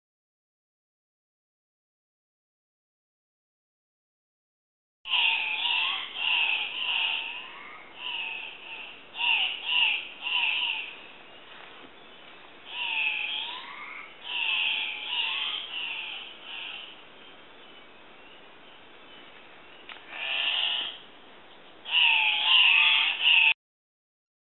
COCK OF THE ROCK MINDO ECUADOR, CLOUD FOREST MINDO ECUADOR BIRD WATCHERS MINDO
COCK OF THE ROCK IS THE SYMBOLIC BIRD OF MINDO. AT THIS RESERVE YOU CAN VIEW OVER 450 BIRD SPECIES IN THEIR NATURAL HABITAT.
BIRD WATCHING WELCOME
COCK OF THE ROCK OF MINDO